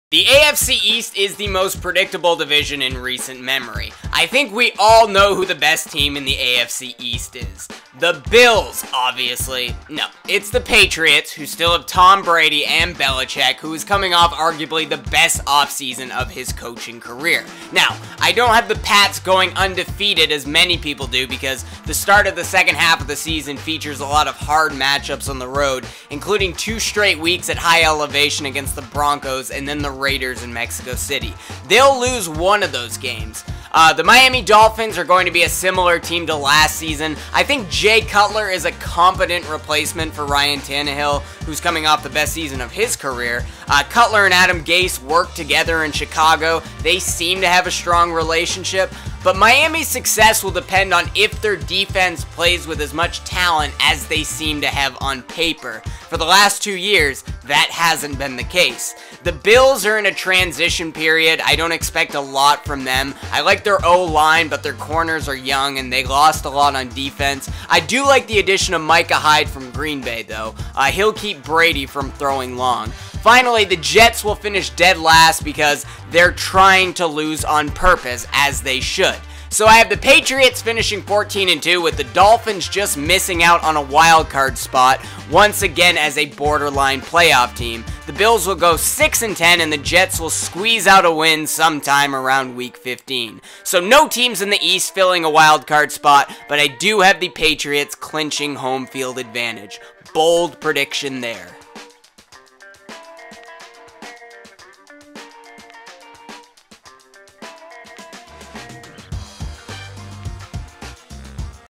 Patriots, Dolphins, Bills, Jets | AFC East Predictions
Can anyone dethrone the Patriots in the AFC East (hint: no). But can the Dolphins make the playoffs with Jay Cutler?